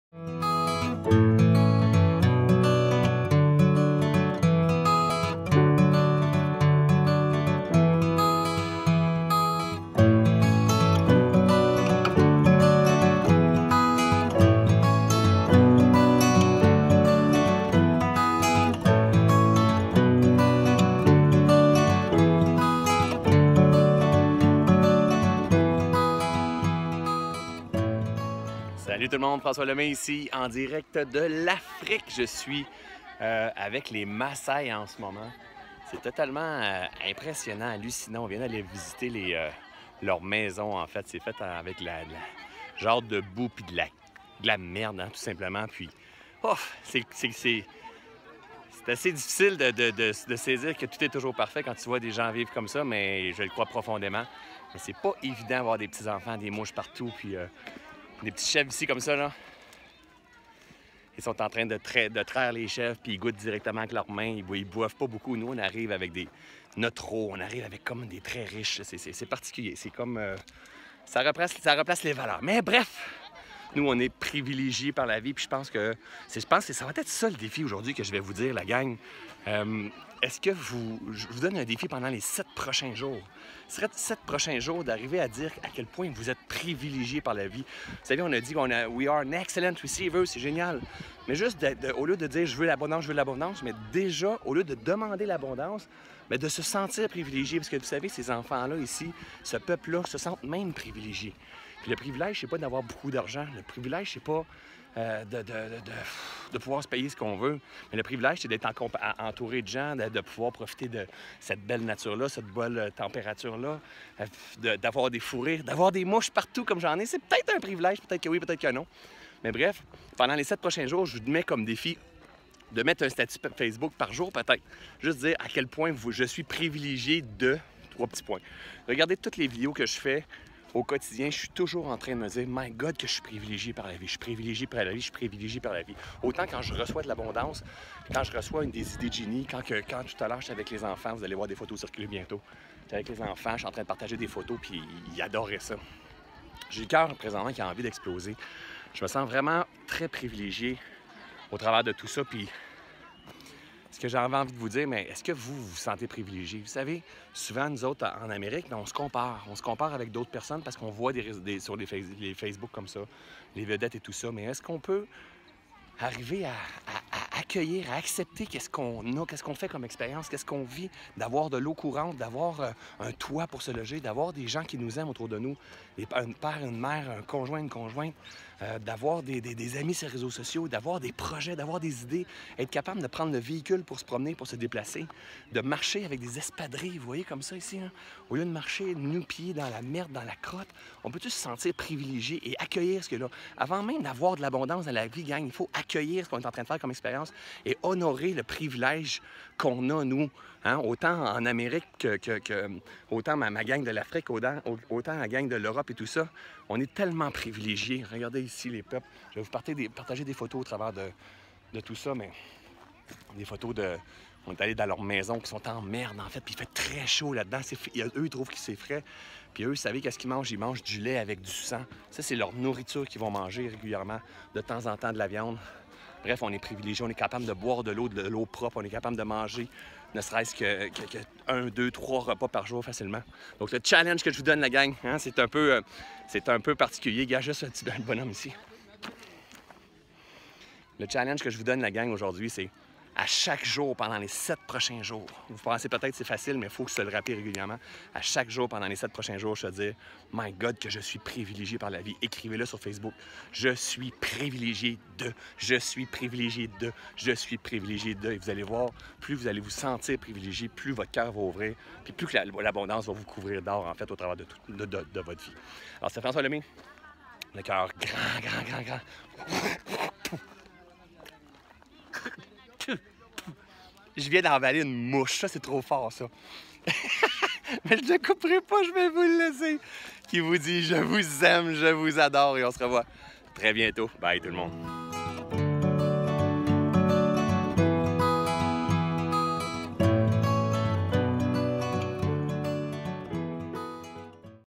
Soyons privilégié [Défi 5] Soyons le changement
Pour en savoir plus sur les différents services offert par François  » »

Pour ce défi, j’étais en direct de l’Afrique avec les Massai. C’est totalement impressionnant. On vient d’aller visiter leurs maisons. C’est fait en boue! Ouf… c’est difficile de saisir que tout est toujours parfait quand tu vois des gens vivre comme ça, mais je le crois profondément. Ce n’est pas évident de voir de petits enfants avec des mouches partout. Ils sont en train de traire les chèvres et ils ne boivent pas beaucoup. Nous, on arrive avec notre eau, comme si on était très riches. C’est particulier… ça replace les valeurs.

Nous on est privilégié par la vie! Pendant les 7 prochains jours, arrivez à dire à quel point vous êtes privilégié par la vie.

On a dit que « We are excellents receivers ». C’est génial! Mais en plus de dire je veux l’abondance, au lieu de demander l’abondance sentez-vous privilégié. Vous savez, ces enfants ici, ce peuple, ils se sentent privilégié. Le privilège ce n'est pas d’avoir beaucoup d’argent, de pouvoir se payer ce qu’on veut. Le privilège c’est d’être entouré de gens, de pouvoir profiter de cette belle nature, belle température, d’avoir des fous rires!

Pendant les 7 prochains jours je vous donne comme défi de mettre un statut Facebook par jour. Dites à quel point je suis privilégié de (…)

Regardez toutes les vidéos que je fais au quotidien. Je suis toujours en train de me dire que je suis privilégié par la vie. J’ai le cœur présentement qui a envie d’exploser. Je me sens vraiment très privilégié au travers de tout ça puis ce que j’avais envie de vous dire c’est : est-ce que vous vous sentez privilégié?

Souvent en Amérique on se compare. On se compare avec d’autres personnes. Est-ce qu’on peut arriver à accueillir, accepter ce qu’on a, ce qu’on fait comme expérience, ce qu’on vit! D’avoir l’eau courante, un toit pour se loger, d’avoir des gens qui nous aiment autour de nous! Un père, une mère, un conjoint, une conjointe, d’avoir des amis, d’avoir des projets, des idées! Être capable de prendre le véhicule pour se promener, pour se déplacer, de marcher avec des espadrilles! Au lieu de marcher nu pieds dans la merde.

Peut-on se sentir privilégié et accueillir ce qui est là. Avant même d’avoir de l’abondance dans la vie, il faut accueillir ce qu’on est en train de faire comme expérience et honorer le privilège qu’on a.

Regardez les peuples ici! On est allé dans leurs maisons qui sont en boue. Eux, ils trouvent que c’est frais, mais c’est très chaud! Et vous savez ce qu’ils mangent? Ils mangent du lait avec du sang. C’est ce qu’ils mangent régulièrement. De temps en temps, ils mangent de la viande. Bref, on est privilégié. On est capable de boire de l’eau, de l’eau propre, de manger ne serait-ce que 1-2-3 repas par jour facilement!

Donc votre défi cette semaine : à chaque jour pendant les 7 prochains jours, vous penserez peut-être que c’est facile mais il faut se le rappeler régulièrement, se dire qu’on est privilégié. Écrivez-le sur Facebook. Je suis privilégié de (…)

Vous allez voir, plus vous allez vous sentir privilégié, plus votre cœur va ouvrir et plus l’abondance va vous couvrir d’or au travers de votre vie.

C’est le cœur grand, grand, grand que je vous dis je vous aime, je vous adore! À bientôt tout le monde!